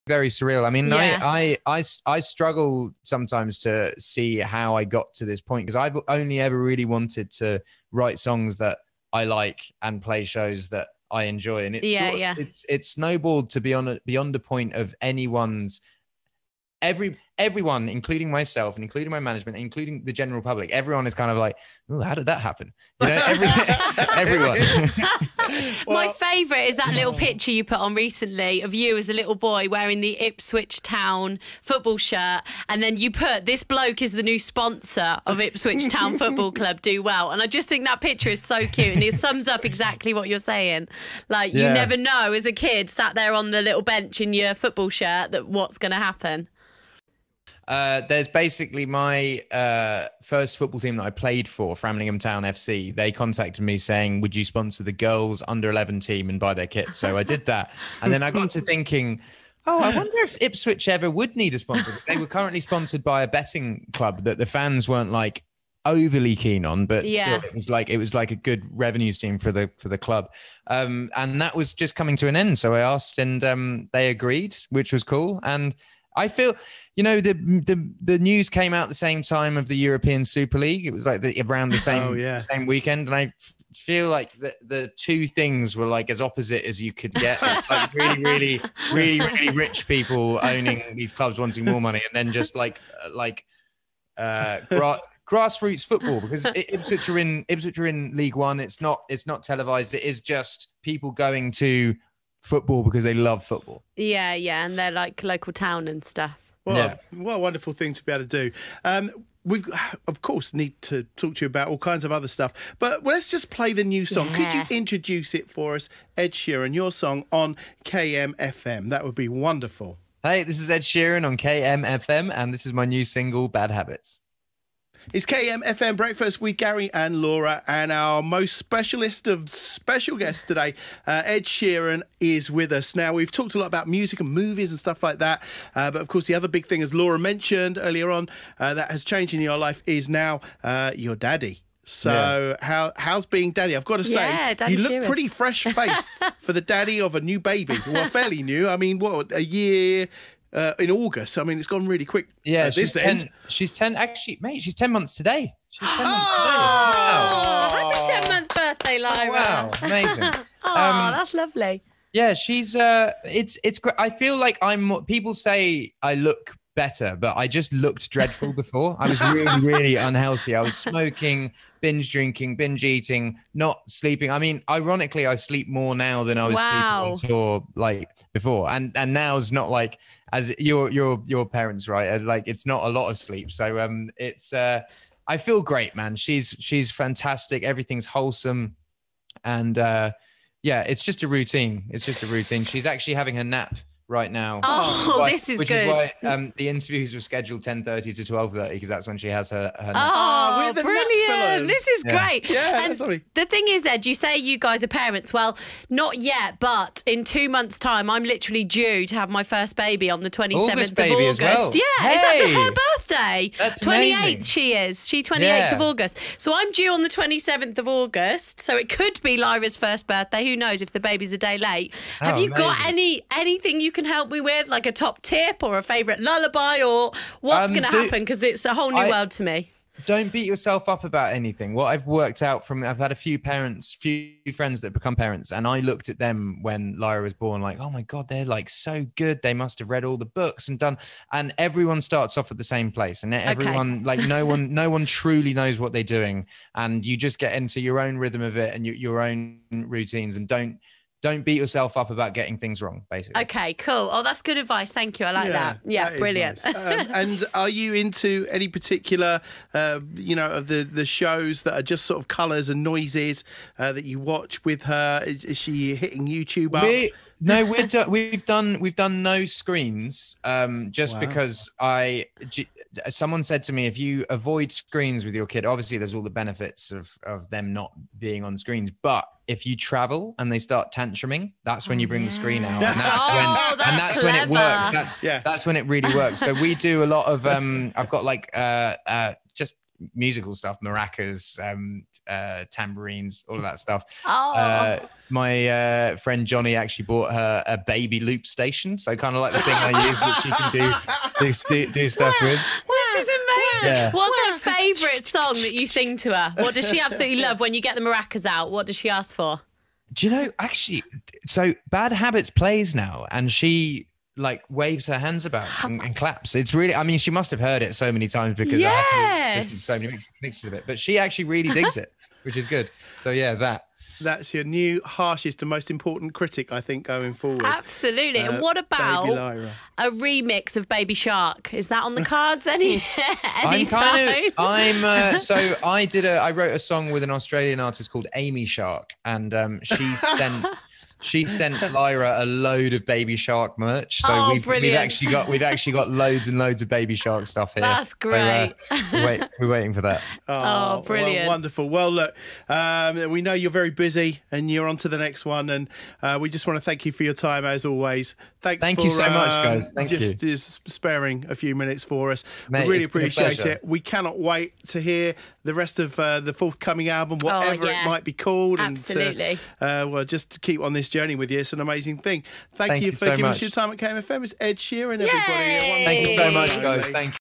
Ed Sheeran speaks about Ipswich Town deal, his Framlingham home and parenthood
Ed Sheeran has been speaking Suffolk News' sister radio station in Kent kmfm about his new single, his love of Framlingham and Ipswich Town and parenthood.